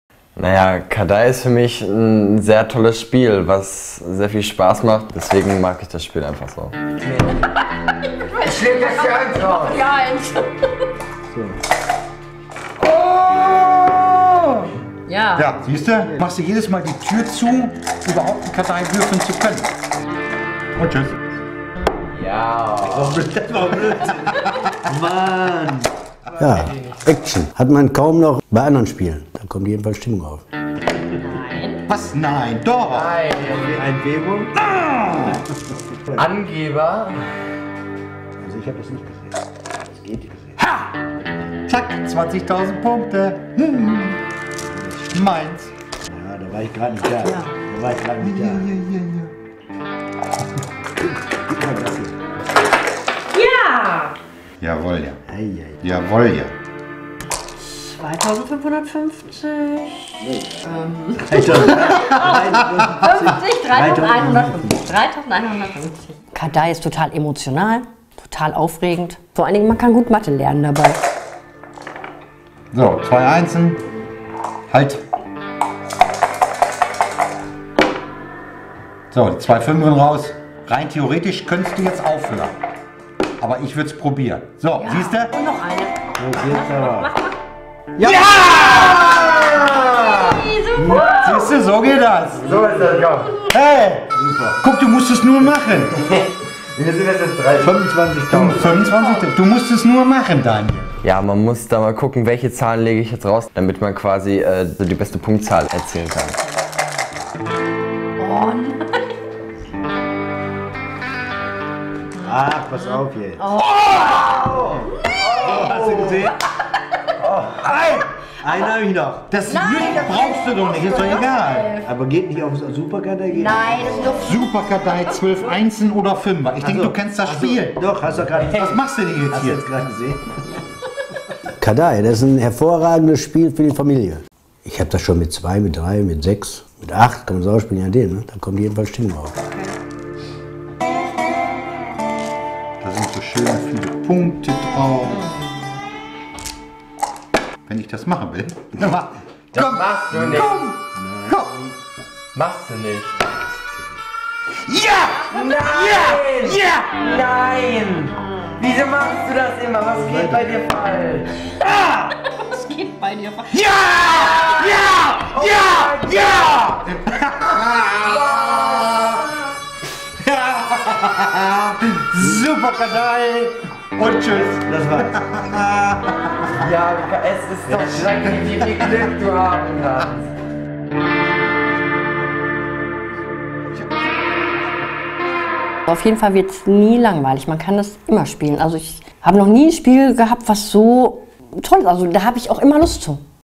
KARDEY ...und Sie werden nicht mehr davon lassen können
KARDEY - Der Film. KARDEY - Das Würfelspiel der Superlative.
D&K Spiele. Noch nie war ein Würfelspiel so einfach und doch so spannend. Durch die vielen verschiedenen Möglichkeiten zu gewinnen, entsteht eine nie da gewesene Spieldynamik, die herausfordert und so bei großem Spielspaß die mentalen Fähigkeiten fördert. Strategie oder Risiko - beide Wege können zum Sieg führen! Für welchen entscheiden Sie sich? Initiator des KARDEY Spiels: Michael Deyda.
Video: VideeO.TV Harald Ortlieb November 2016